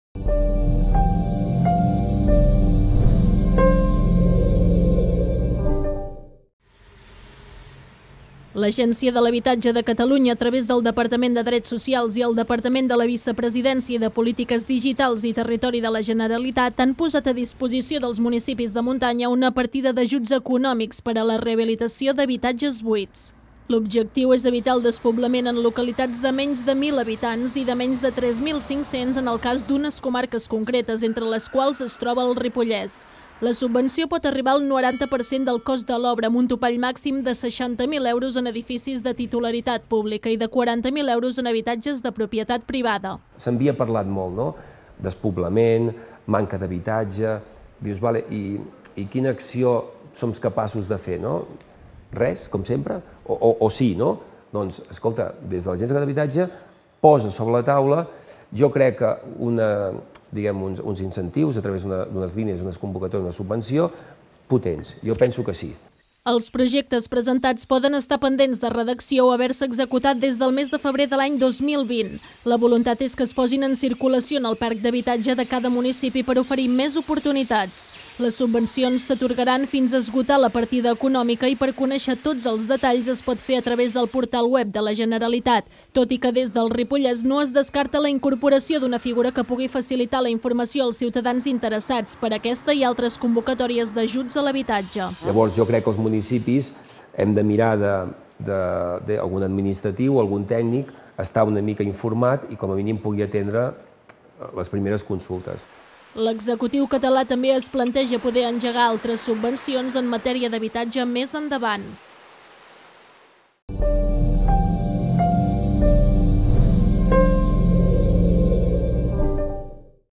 El Ripollès opta a 5 milions d'euros per a rehabilitar habitatge
L'Agència de l'Habitatge de Catalunya posa a disposició dels municipis de muntanya una partida d'ajuts econòmics per a la rehabilitació d'habitatges buits. L'objectiu és evitar el despoblament en localitats de menys de 1.000 habitants, i de menys de 3.500 en el cas d'unes comarques concretes, entre les quals es troba el Ripollès. La subvenció pot arribar al 90% del cost de l'obra, amb un topall màxim de 60.000 euros en edificis de titularitat pública i de 40.000 euros en habitatges de propietat privada.